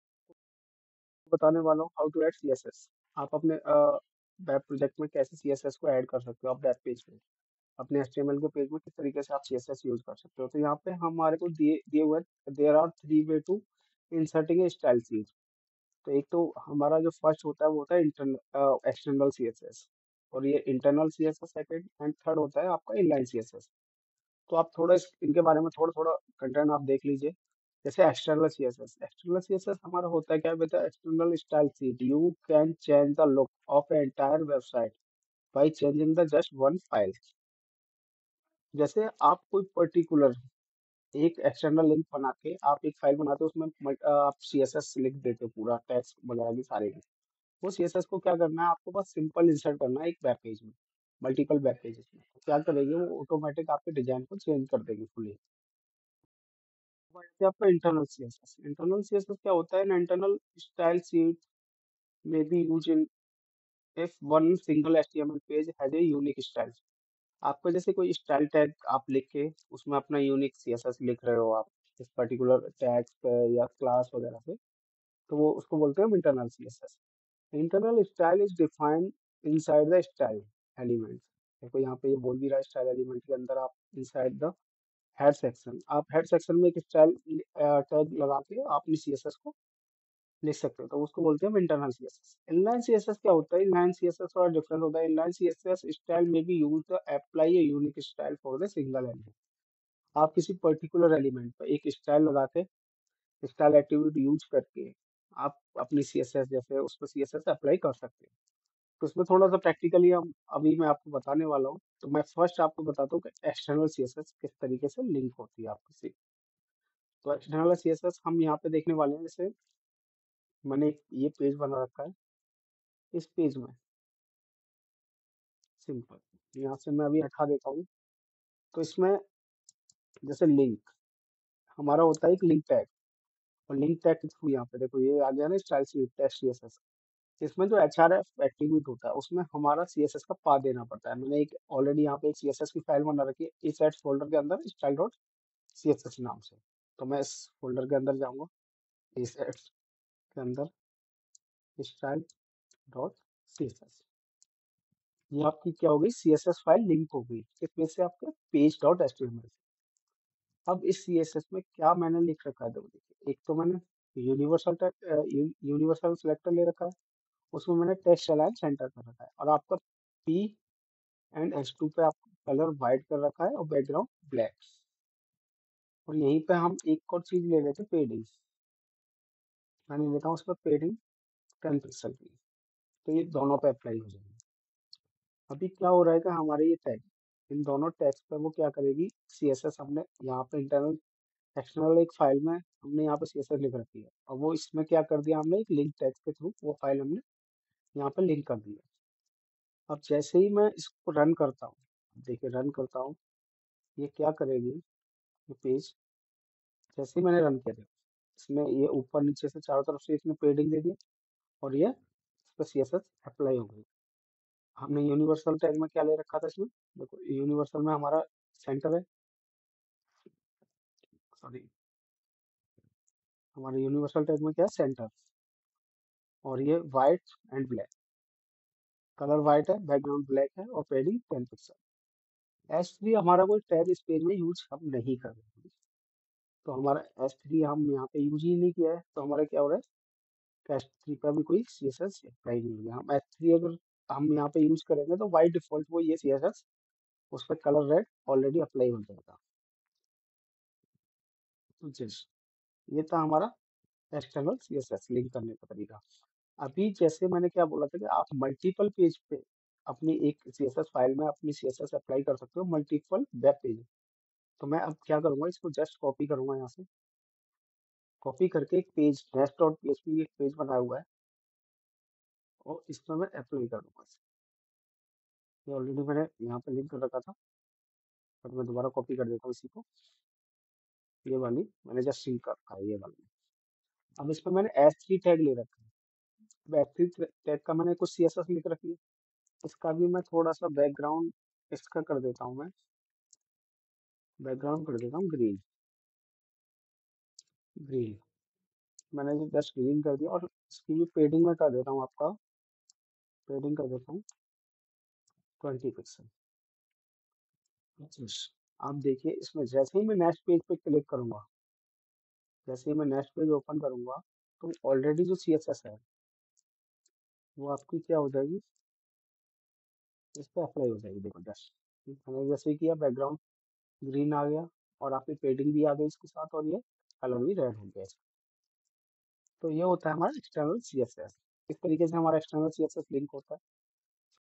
Lecture 4 - External CSS
About this video : - With an external style sheet, you can change the look of an entire website by changing just one file!

Each HTML page must include a reference to the external style sheet file inside the link element, inside the head section.